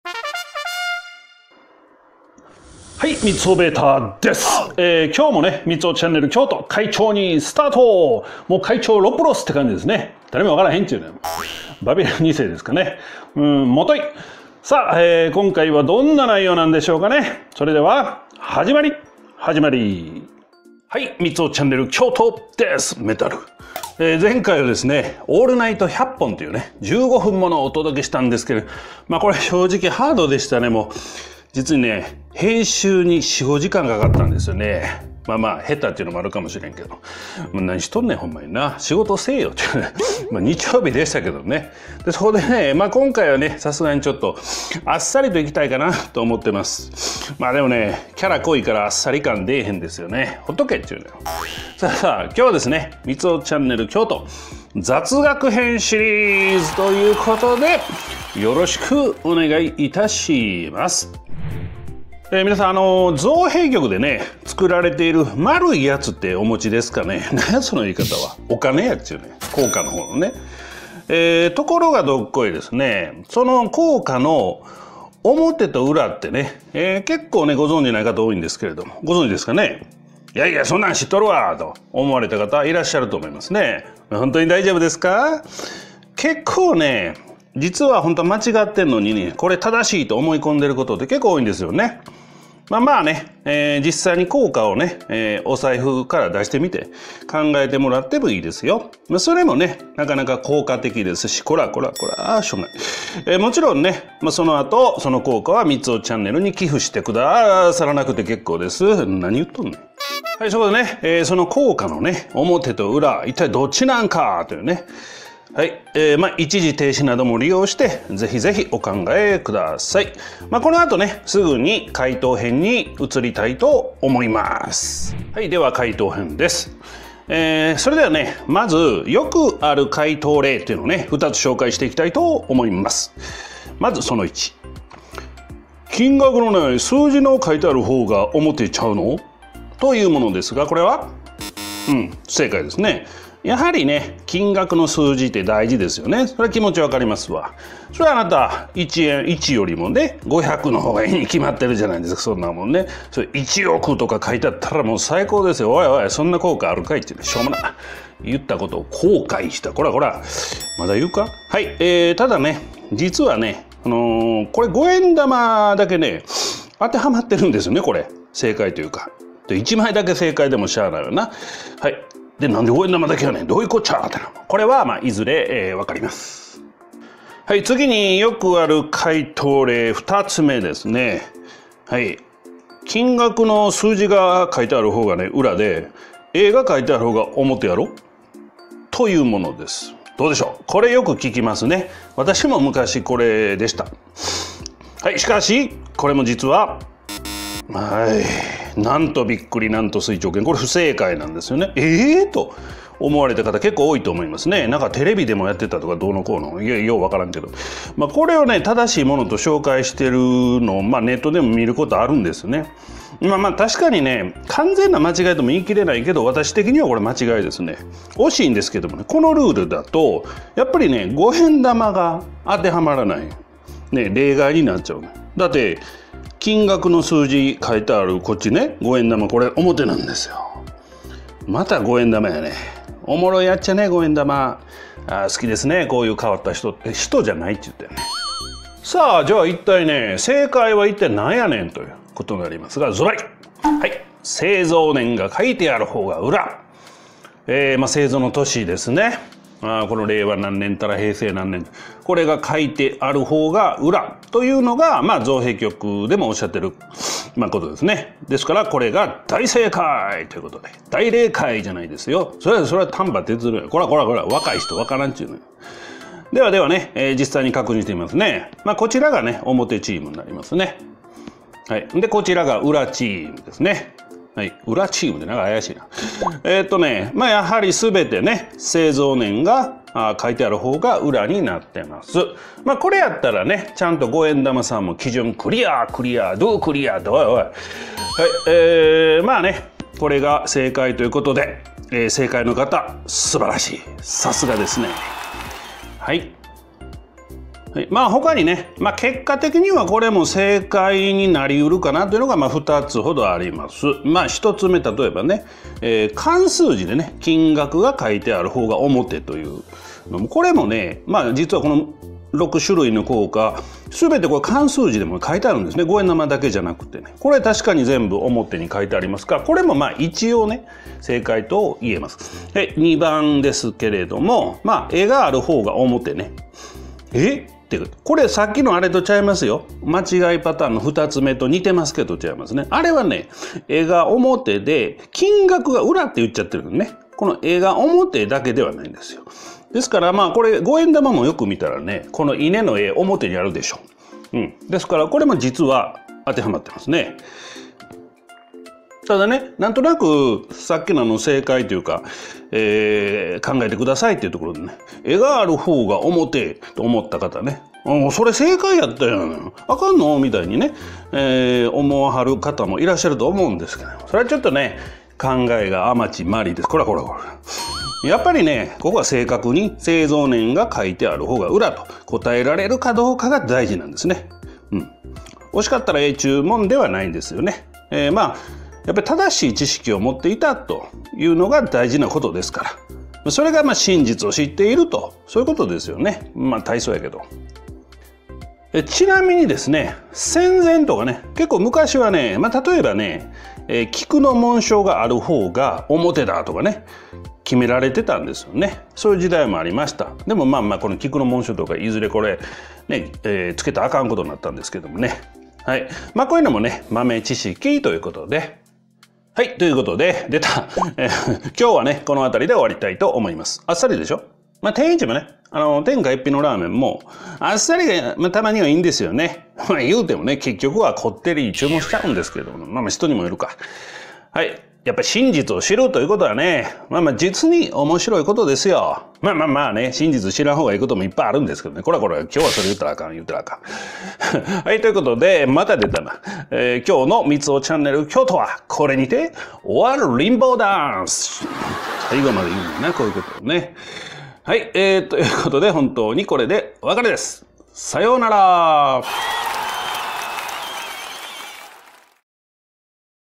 硬貨の表と裏を正確に指摘出来ますか？とにかくご覧ください。｢知ってるつもり｣のことって実に多いんです。320ch.京都
雑学編スタート。硬貨の裏と表を正確に指摘出来る方は実に少ないと思います。絵が表？本当ですか？硬貨の表と裏